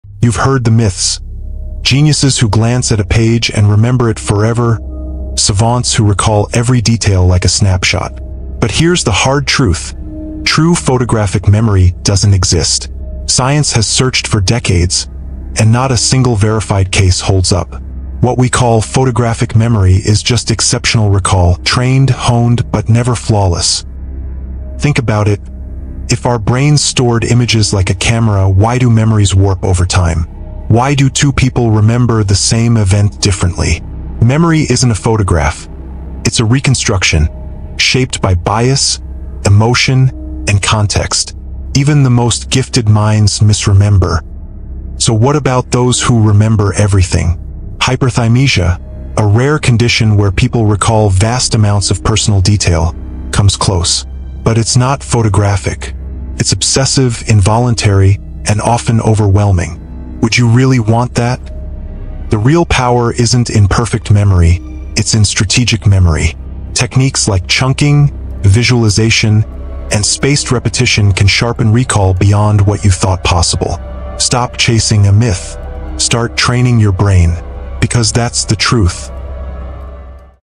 The Truth About Photographic Memory—Does It Exist?
Is photographic memory real, or just a myth? Science reveals the shocking truth about perfect recall—and why your brain doesn’t work like a camera. Discover the difference between real memory champions and Hollywood myths. Learn powerful memory techniques to boost retention, enhance focus, and train your brain like the pros. Forget what you think you know—this is the real science of memory!